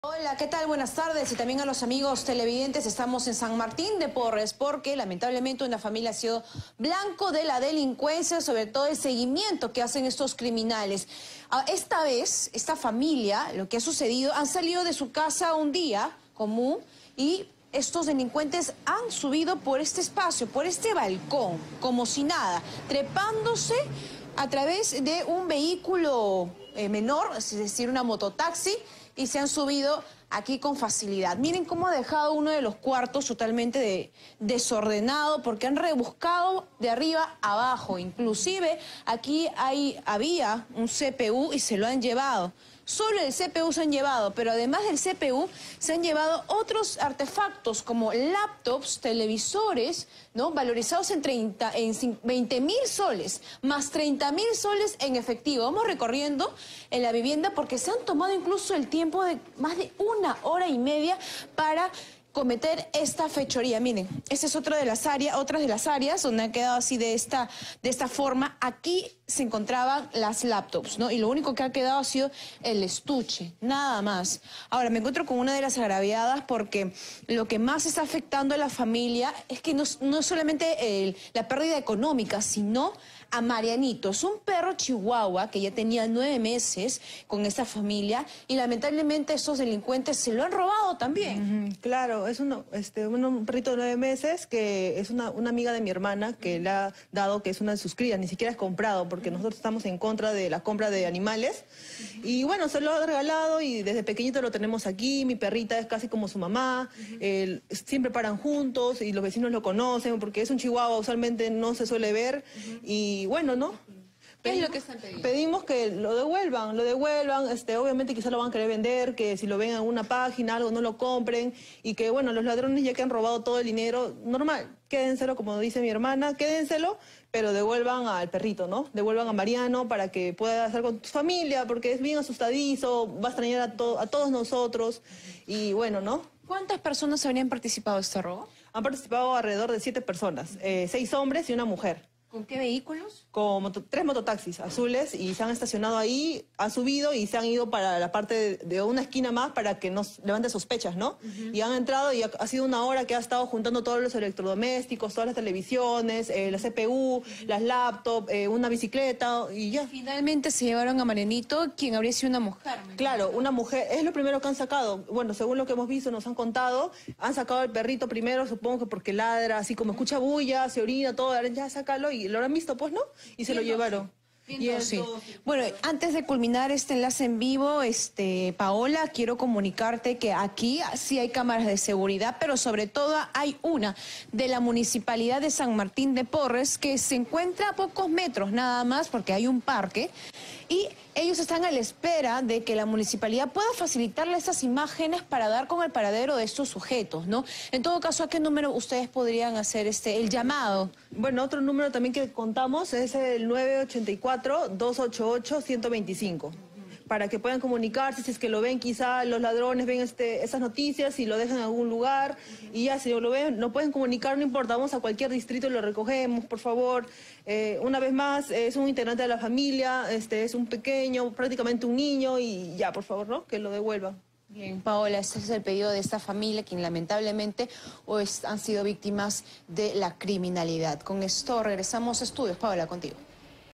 SMP: delincuentes roban más de S/30 mil y se llevan hasta el perrito de la familia
La inseguridad ciudadana continúa aumentando en nuestro país. Esta vez, un grupo de delincuentes ingresaron a una vivienda en San Martín de Porres (SMP) para robar más de S/30 mil y como si no fuera poco se llevaron el perrito de la familia.

HECHOS

El predio cuenta con un balcón, punto que fue aprovechado por los criminales para ingresar al domicilio después de utilizar la parte trasera de la mototaxi y una vez dentro del hogar se apoderaron de todo a su paso.

En declaraciones, una de las víctimas del atraco hizo un llamado a los hampones para que les puedan devolver el perrito de nombre “Mariano”, mascota al cual lo tienen desde hace nueve meses.

De igual manera, contó que en este robo participaron cerca de siete personas en las que se encontraron seis varones y una mujer, todos los criminales llegaron hasta la vivienda a bordo de tres mototaxis.

Finalmente, la familia de este robo pidió a las autoridades de la municipalidad de SMP para entregar las imágenes de las cámaras de seguridad que se localizan a pocos metros de la vivienda.